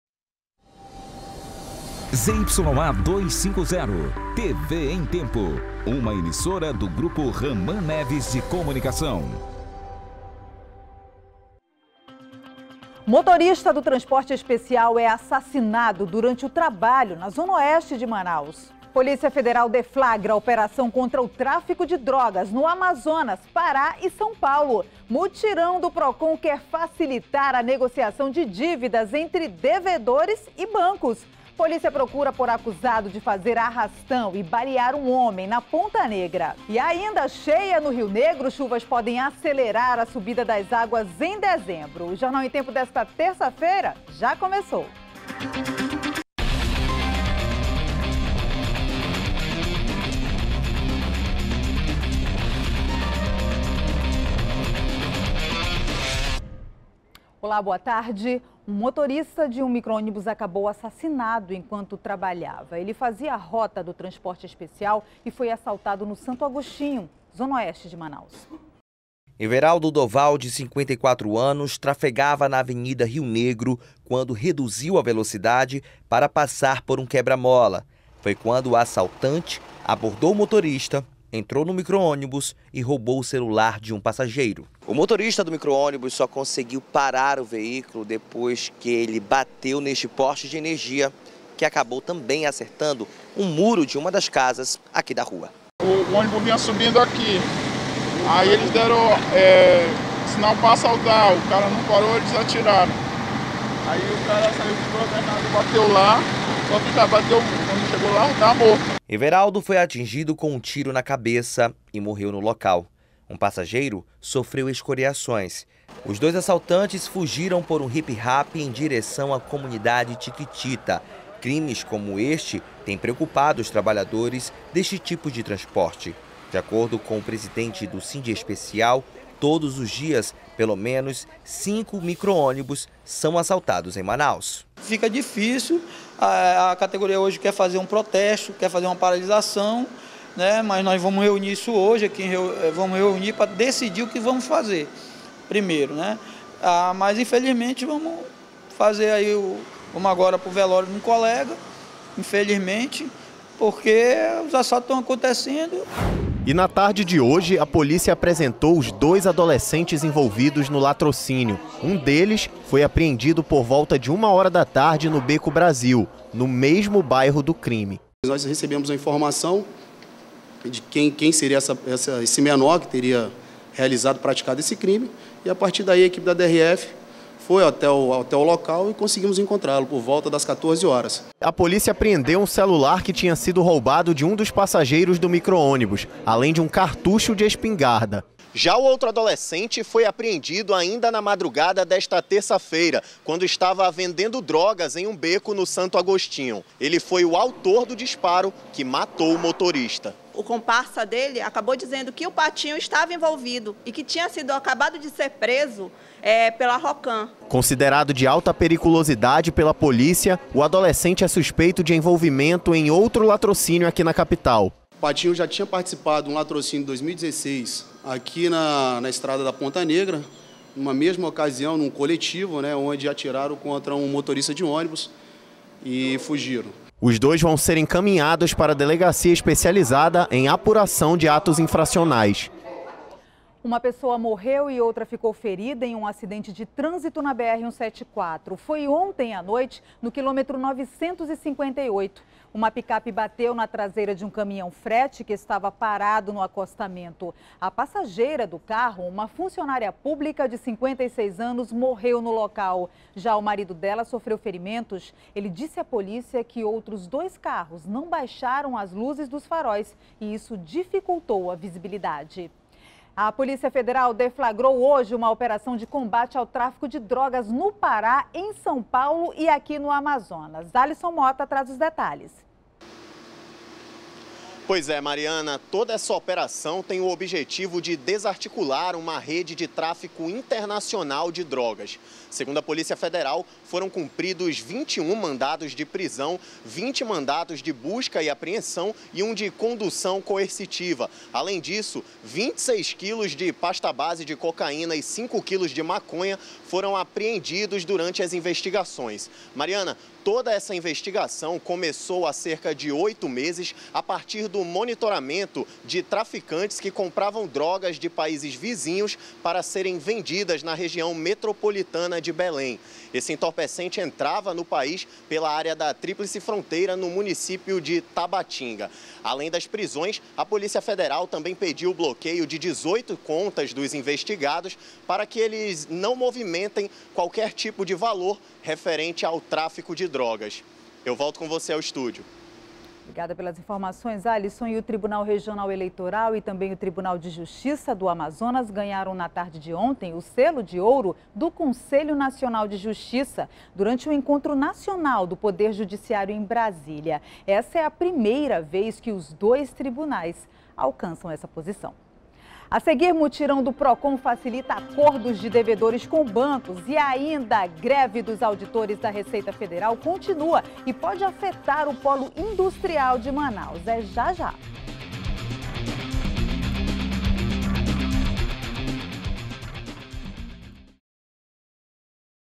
Motorista do transporte especial é assassinado durante o trabalho na zona oeste de Manaus
01º BLOCO JORNAL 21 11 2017 - Motorista do transporte especial é assassinado durante o trabalho na zona oeste de Manaus